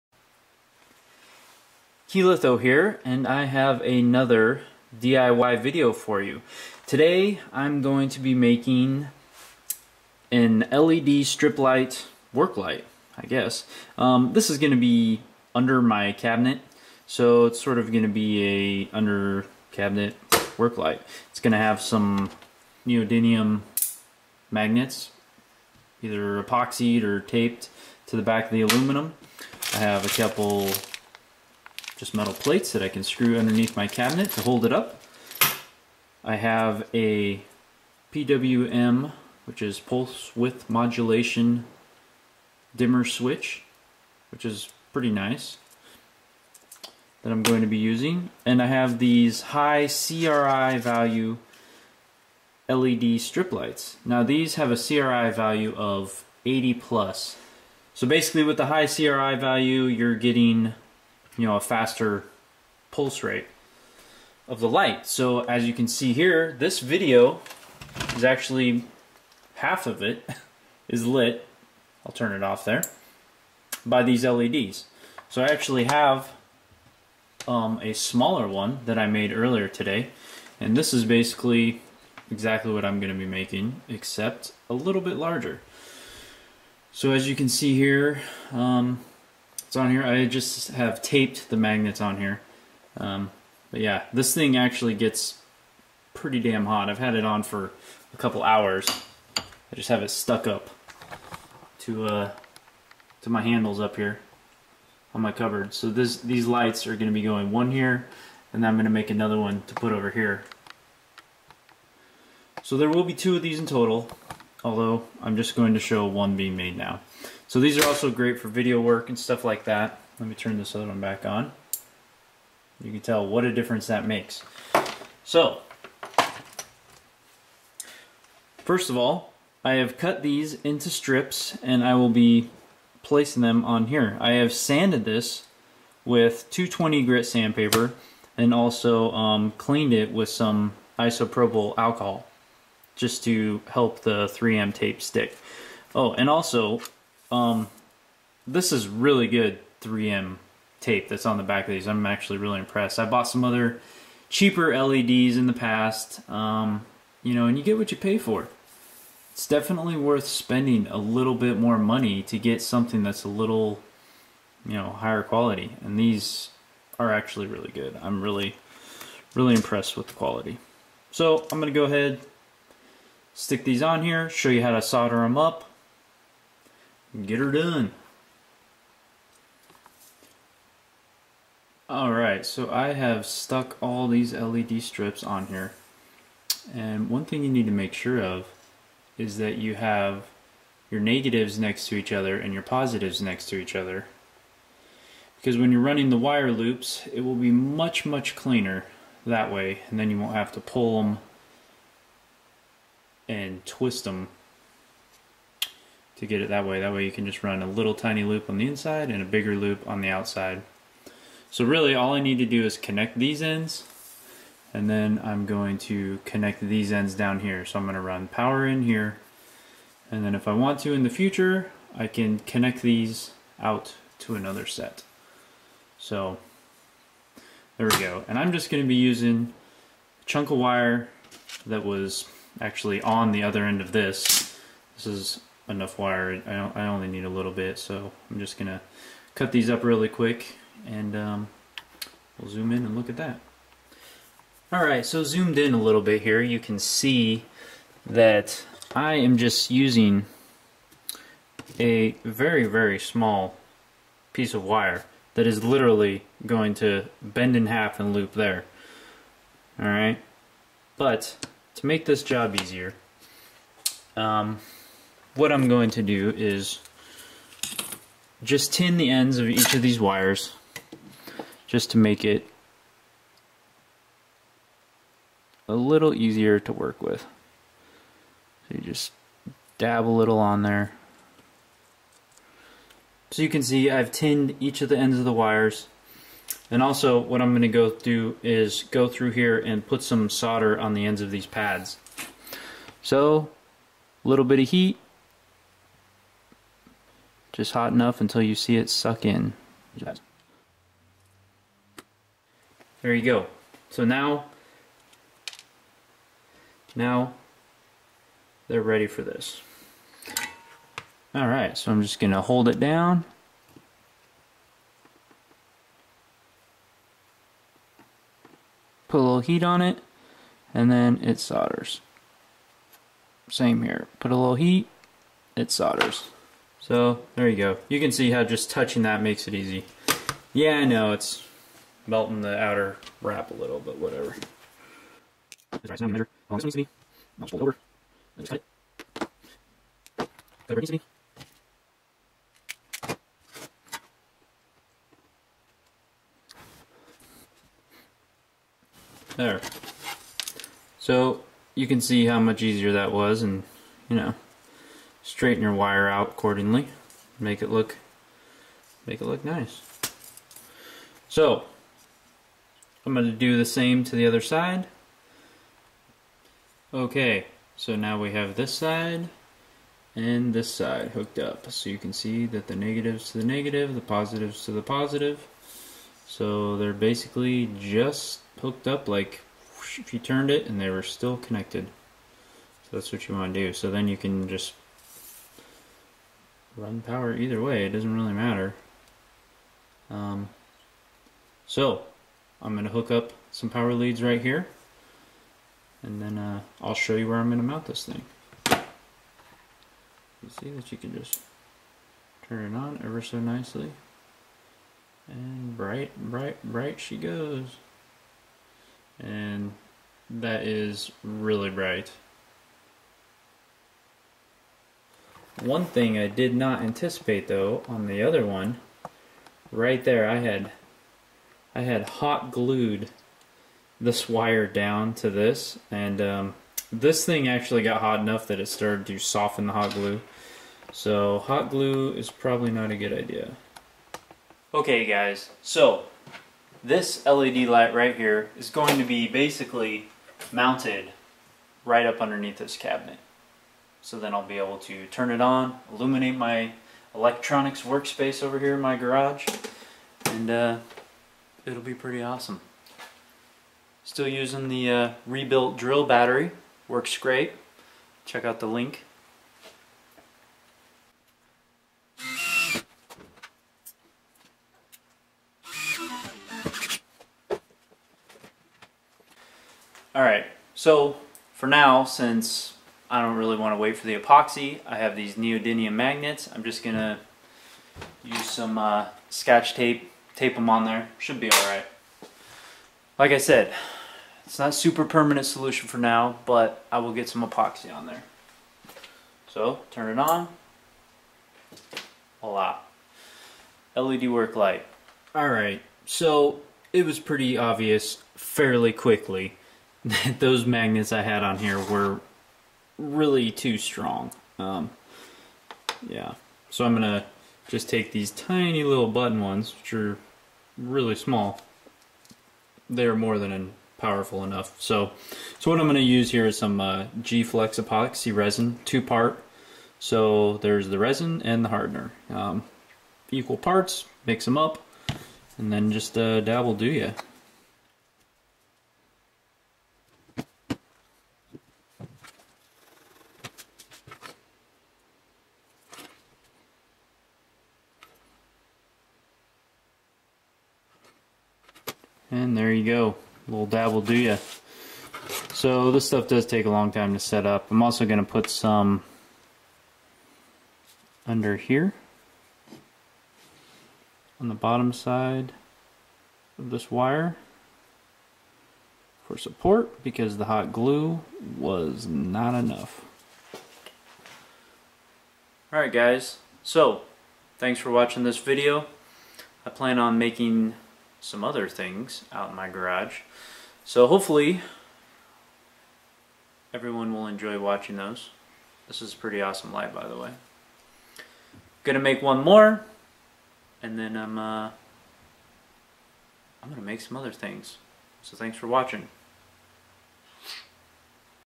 DIY LED Video light / Work light
Making an LED Light Bar for my workbench.
I am using a 12v 15A bench power supply to power this light.

Led LIghts

PWM Dimmer

Aluminum Bar

G/Flex Epoxy - (Cheaper Epoxy would work just fine)